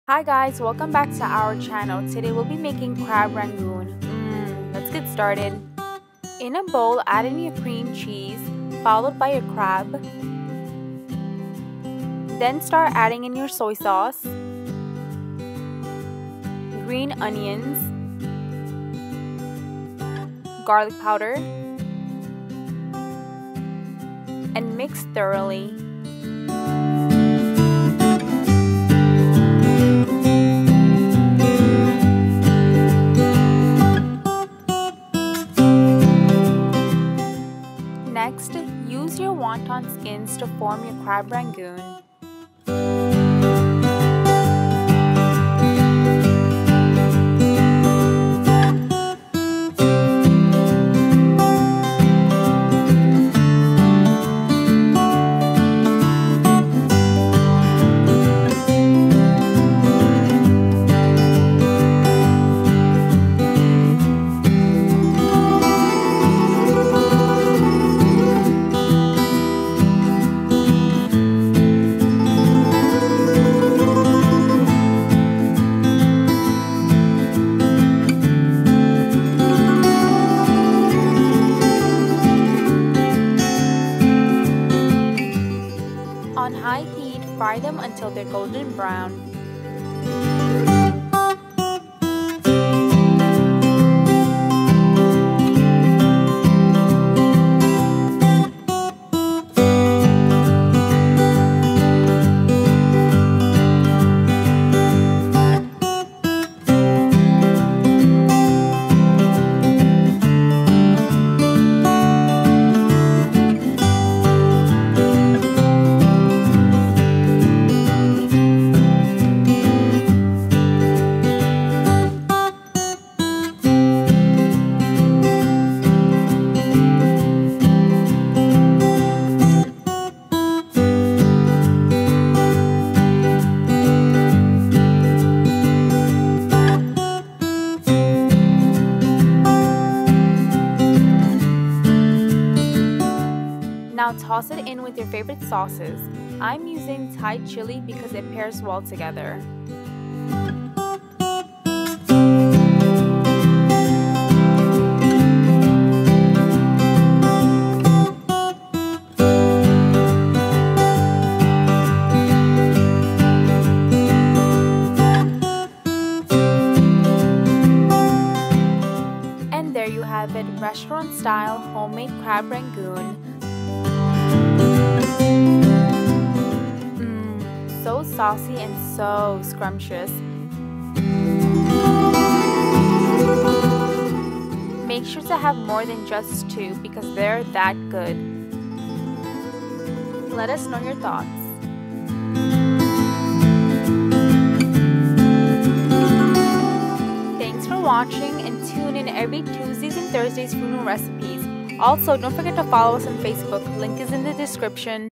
Crispy Sweet Chili Crab Rangoon | Ruma's Rannaghor
Hi Viewers,

The recipe I'll be sharing with you today is a simple appetizer recipe, Crab Rangoon. The first time I tried this was at a Thai restaurant and thought I had to re-create this. It's super quick to make and a great appetizer for any party.

Makes about 25 crab rangoons

4oz cream cheese
5 oz imitation crab
1 ½ tsp soy sauce
2 tbsp scallions
¼ tsp garlic powder

25 Wonton Skins

Fill your skin with the filling, close and fry for 6-7 seconds on each side.

Check us out on Facebook, give us a follow & a like!

Tune in EVERY TUESDAY & THURSDAY for a new video!